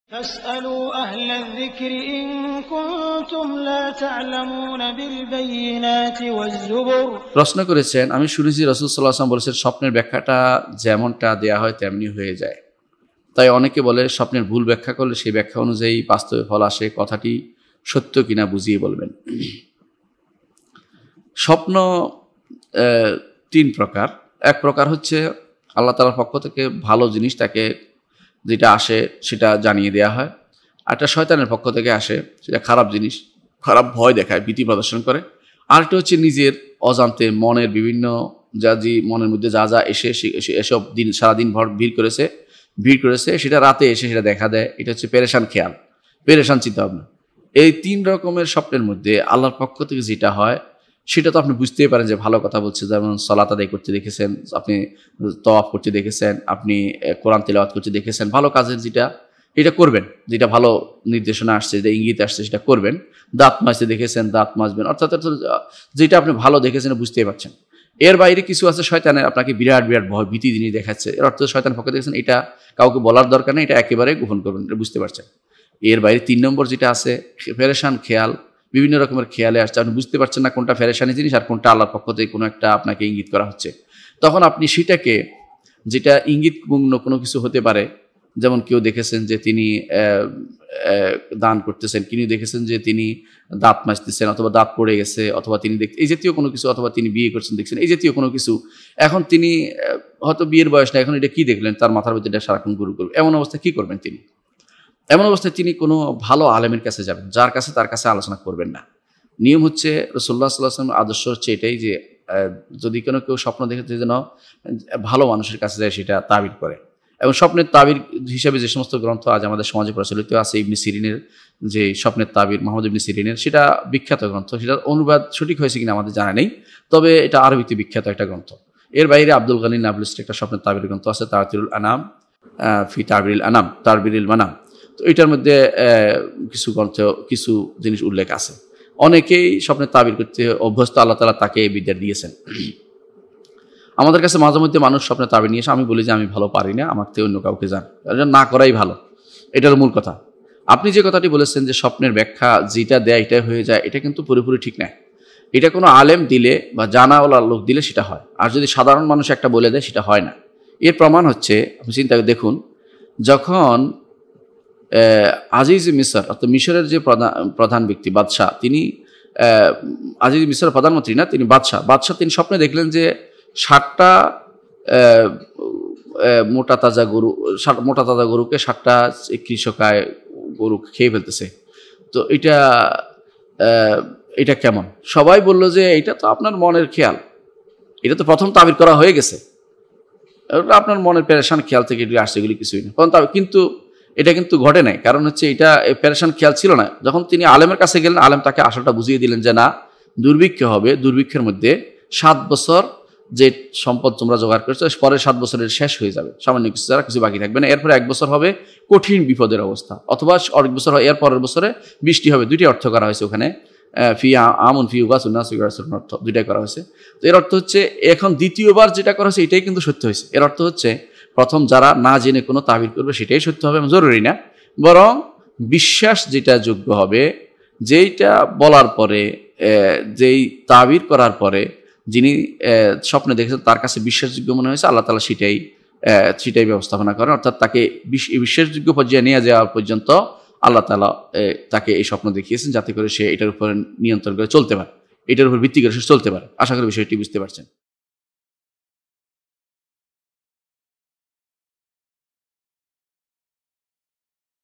প্রশ্ন : স্বপ্নের ব্যাখ্যা কিভাবে করবো? শাইখ প্রফেসর ড. আবু বকর মুহাম্মাদ যাকারিয়া
প্রশ্ন : স্বপ্নের ব্যাখ্যা কিভাবে করবো?
শাইখ প্রফেসর ড. আবু বকর মুহাম্মাদ যাকারিয়া